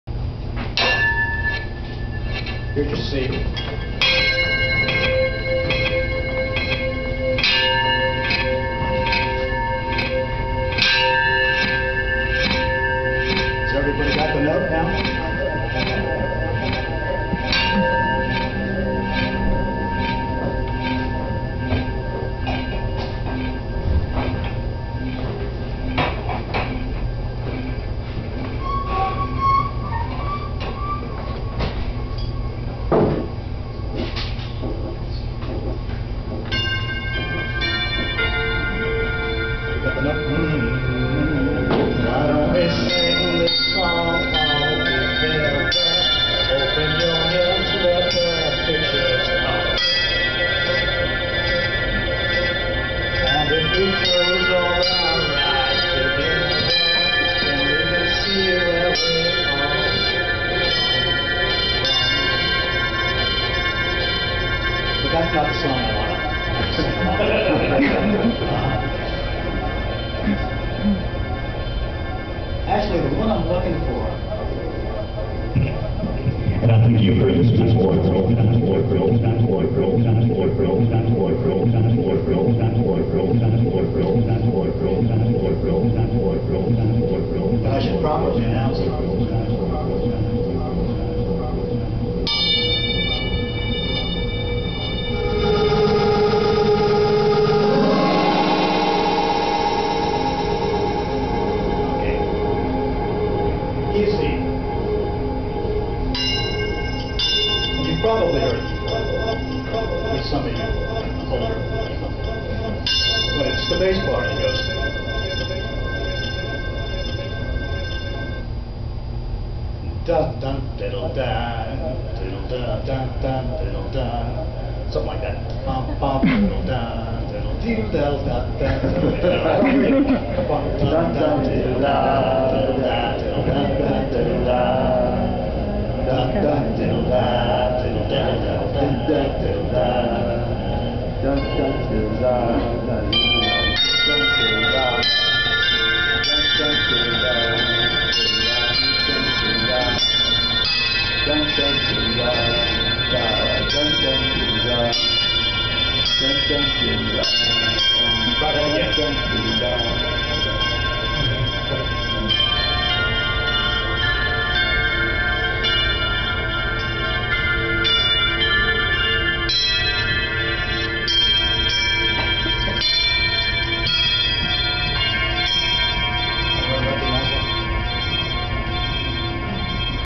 Electric Possible Part 7
The sing-along. Performance by Fast Forty at The Electric Possible, May 13, 2009, at the DC Arts Center in the Adams Morgan neighborhood of Washington, D.C. Also on the bill were the synth duo of Dave Vosh and Logan Mitchell Sr. and the turntable duo of Violet and BLK w / Bear.